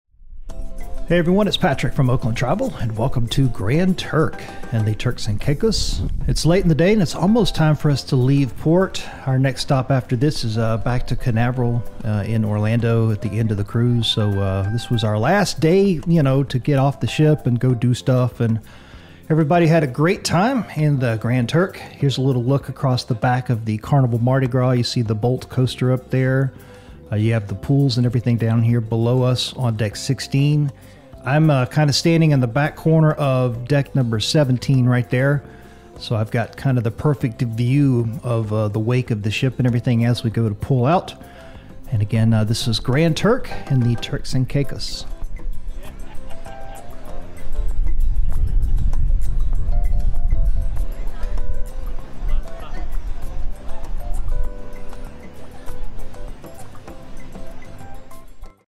Grand Turk In The Turks & Caicos
It's late in the day at Grand Turk and almost time for the Carnival Mardi Gras to leave port and head back to Port Canaveral at the end of the cruise.